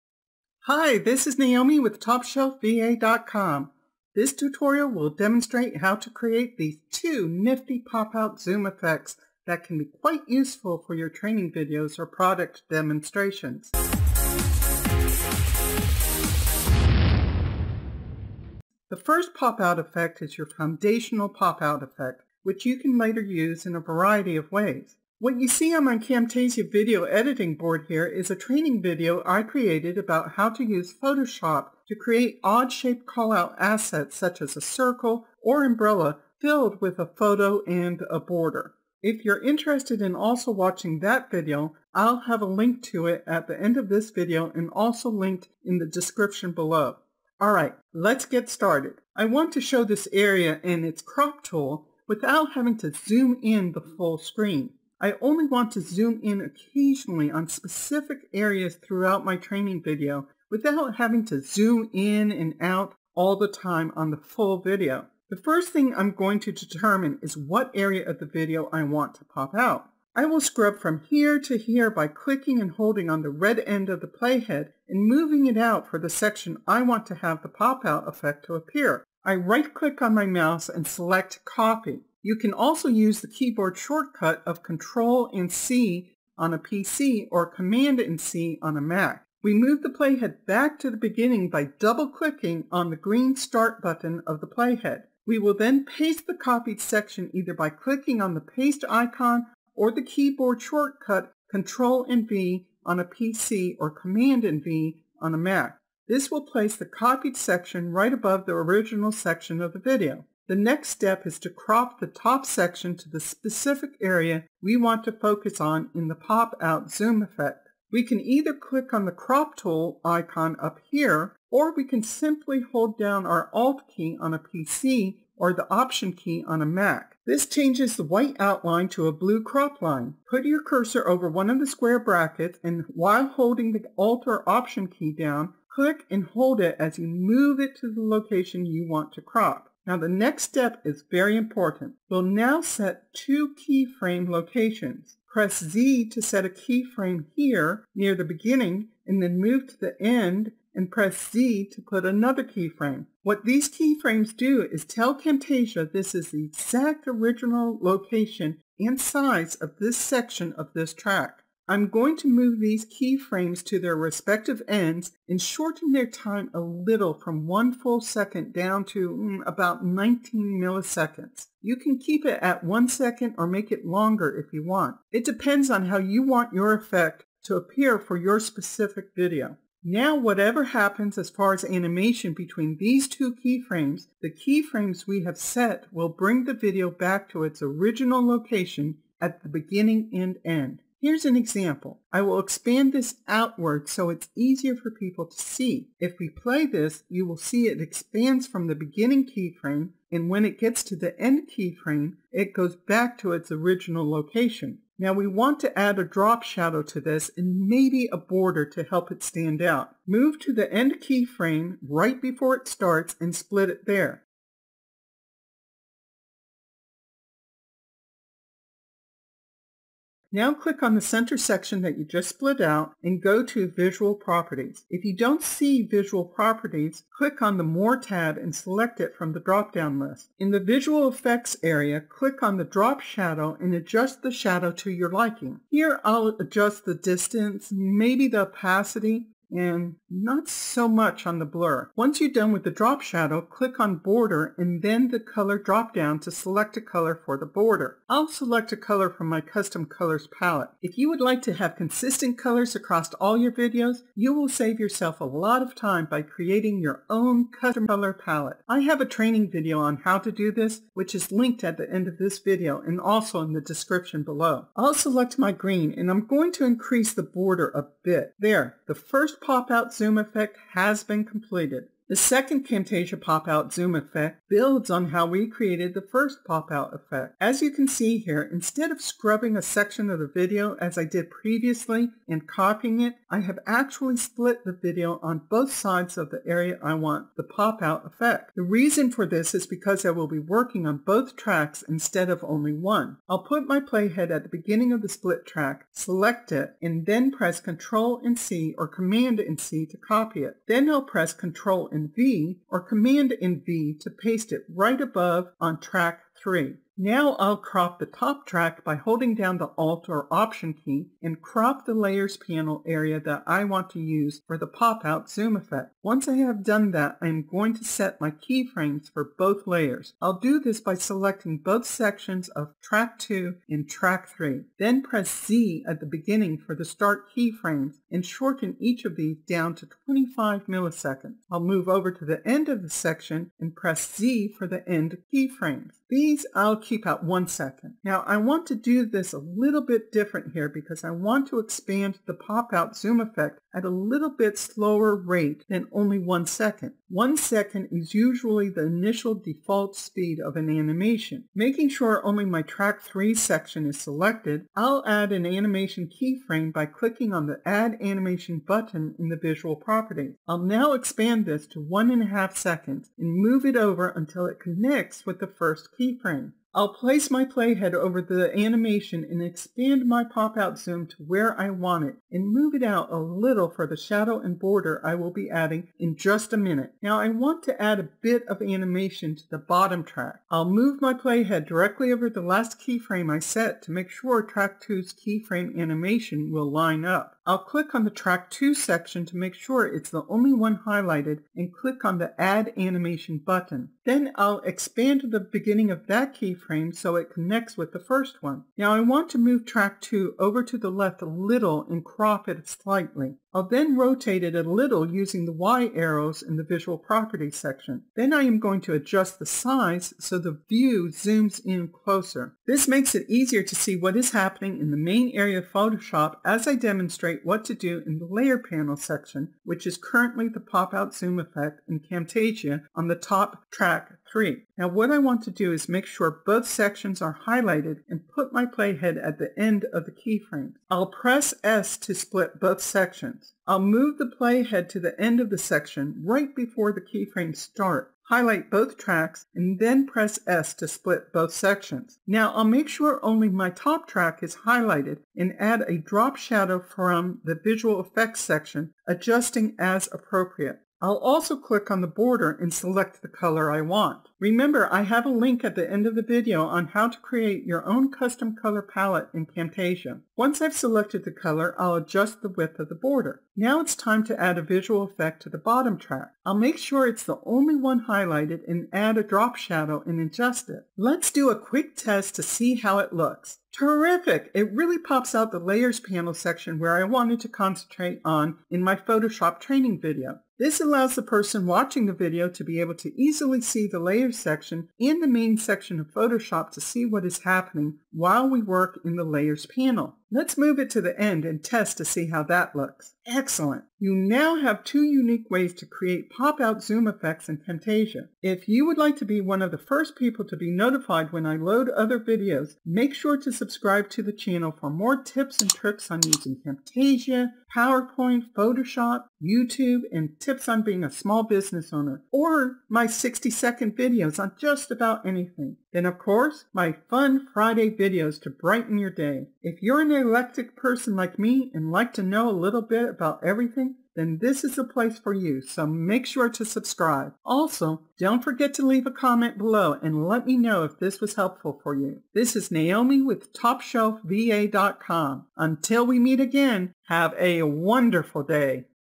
Create Two Camtasia "Pop Out" Zoom Effects in Camtasia Studio
Create Two Camtasia "Pop Out" Zoom Effects in Camtasia Studio: This Camtasia video tutorial will demonstrate how to create two nifty pop out zoom effects that can be quite useful for your training videos or product demonstrations. The first pop out effect is your foundational pop out effect, which you can later use in a variety of ways. (withcaptions

I only want to zoom in occasionally on specific areas throughout my training video, without having to zoom in and out all the time on the full video.  The first thing I am going to determine is what area of the video I want to pop out.

This Camtasia video tutorial includes the main topic of video pop out effects, along with Camtasia hot keys or shortcuts that will help cut down your editing time.  We’ll also discuss setting key frames and how to properly set animations in Camtasia.

If you do training tutorial videos, product demonstration, or software demonstration videos; you will find the pop out zoom effect will greatly enhance your video.  Simply be careful to not over use it as in anything in video editing.  Use judiciously.

Watch the video for the full details on Create Two Camtasia "Pop Out" Zoom Effects in Camtasia Studio.

OTHER VIDEOS REFERRED TO IN THIS VIDEO

Hit the ***LIKE*** button, it really helps!